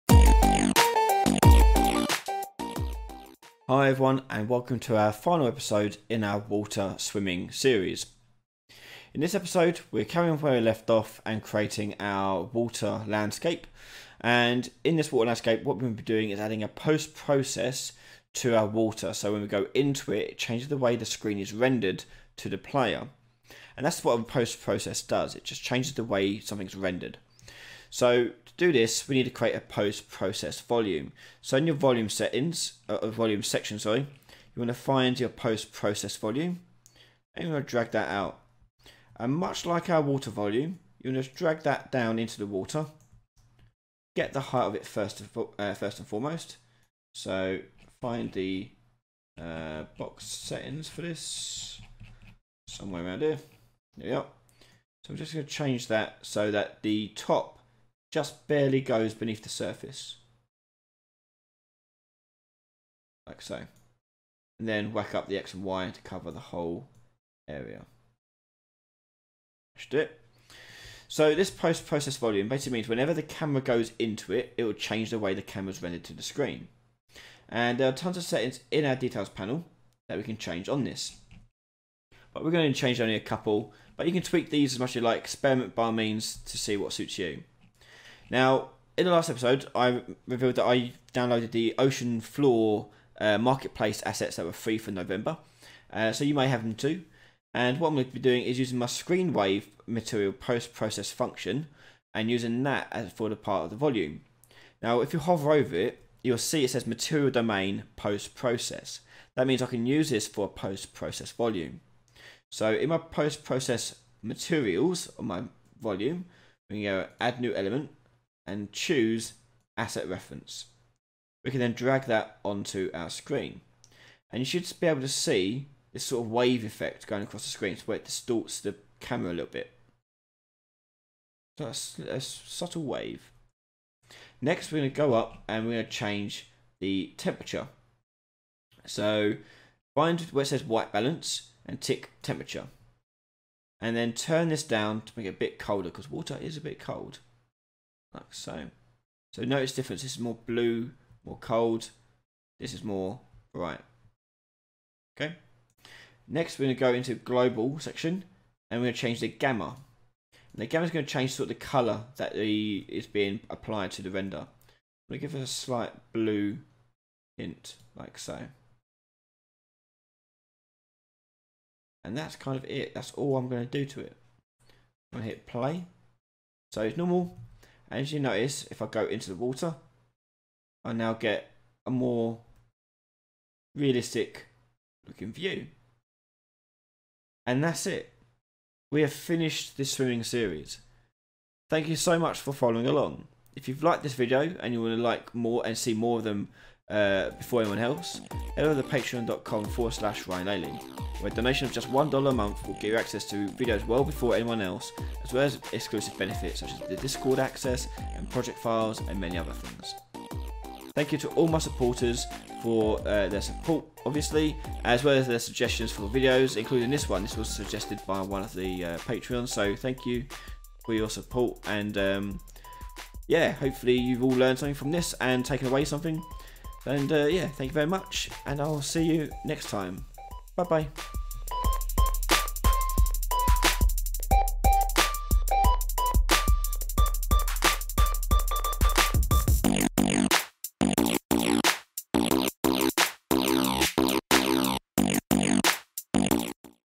Unreal Engine 4 Tutorial - Swimming - Part 5 Post Process
In this short series we go through how to add swimming movement to a third person character. In episode 5 we wrap things up with a post process to add an effect to being under water.